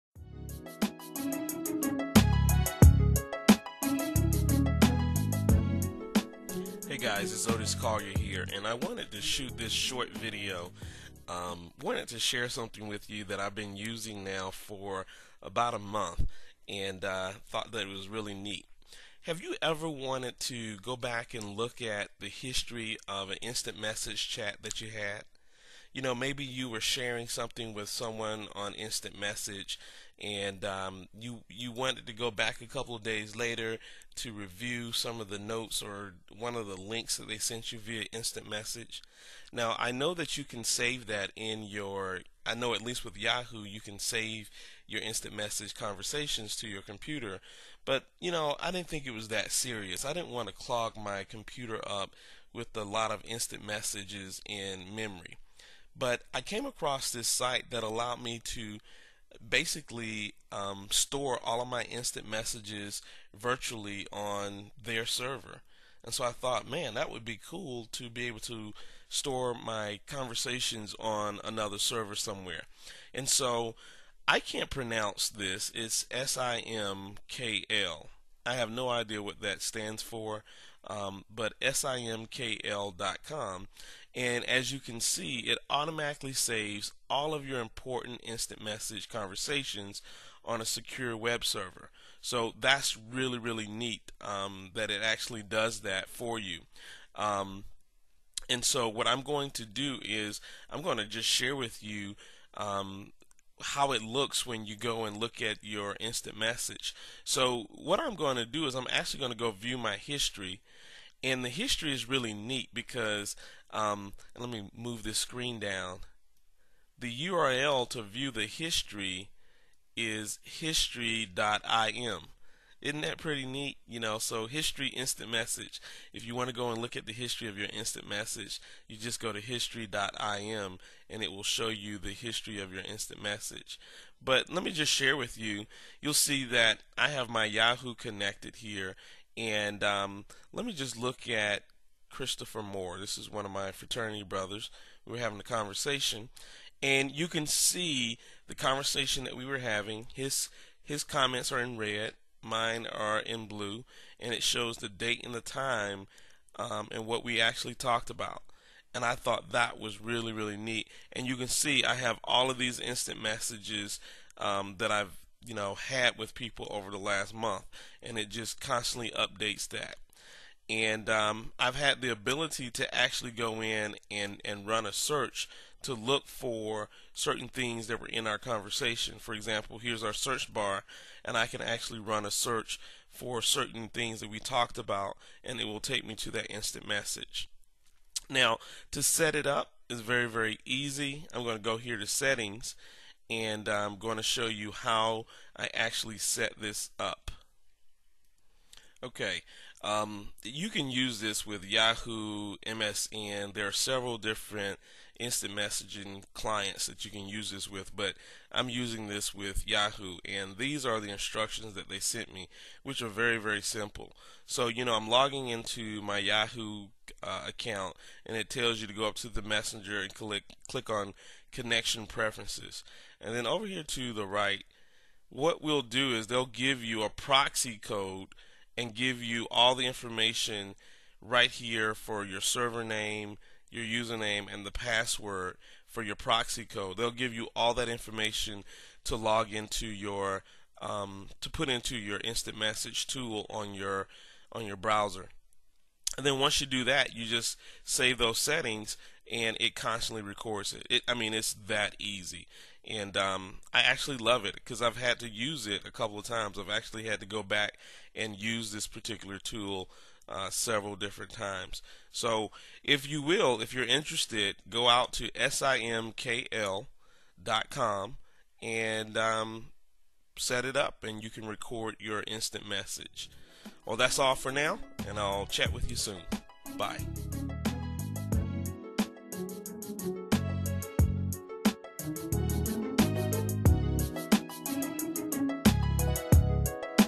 Simkl IM History Saver - record MSN\ICQ\AIM\YAhoo chat logs online
- Simkl automatically saves all of your important IM conversations on a secured web server, the same way you save your e-mails. And more importantly software installation is only needed for Windows Live Messenger. There is no need to install our software to log messages from Pidgin, Digsby, Adium, Miranda, ICQ, and other instant messengers, so no viruses or adware ever. Sign up and you will have easy access to your IM conversations from any computer.